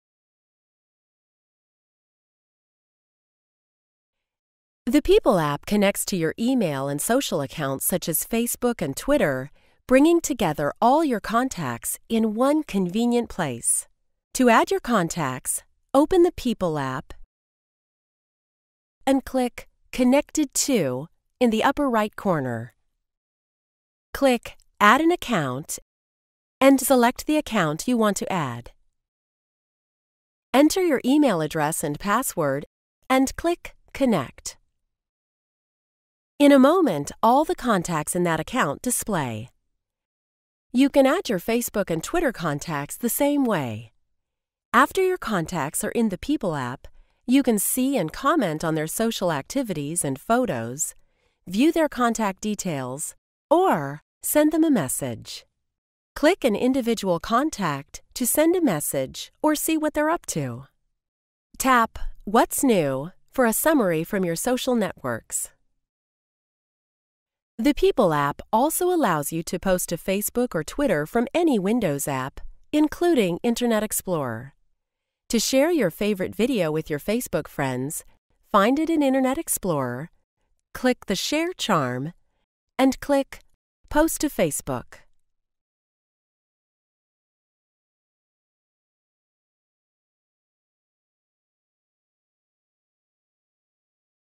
Använda appen Personer i Windows 8.1 för HP-datorer
Med appen Personer kan du ansluta personliga konton och integrera dem med andra appar. Den här videon förklarar hur du ansluter till dina e-post- och sociala konton och hur du snabbt delar onlineinnehåll med appen Personer. Den här filmen producerades av HP.